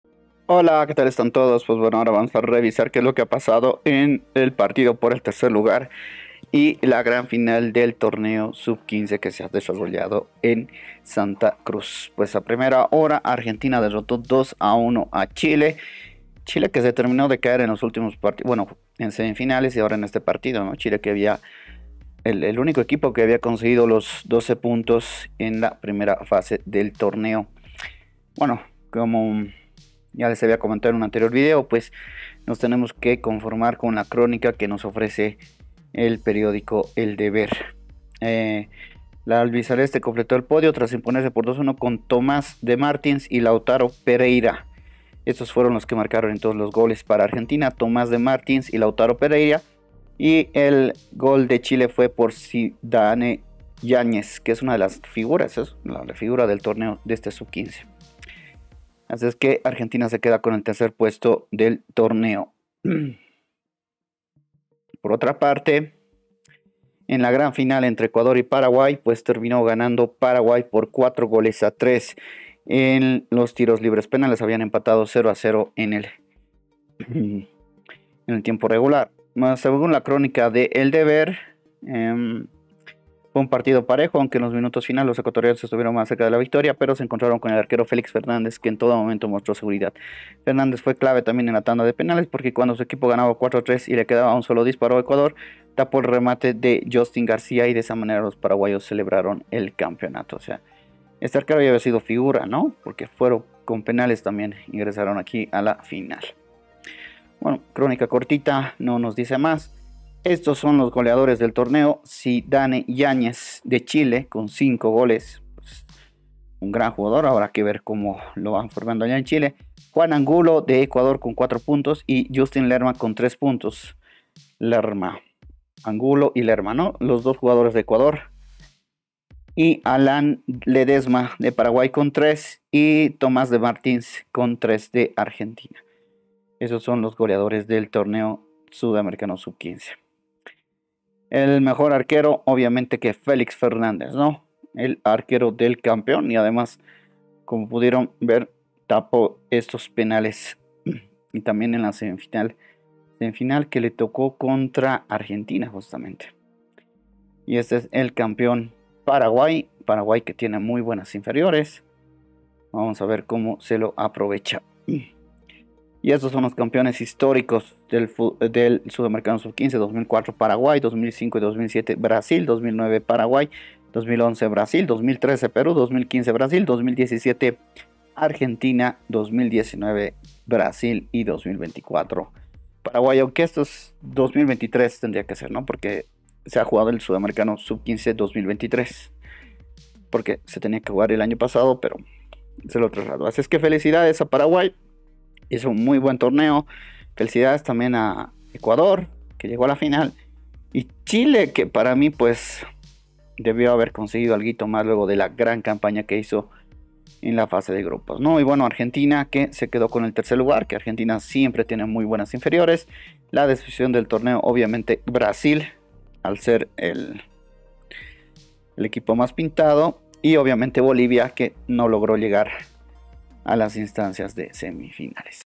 Sudamericano Sub 15 2023, Final: Paraguay Campeón
Los resultados de la final y el partido por el tercer lugar del  Sudamericano Sub 15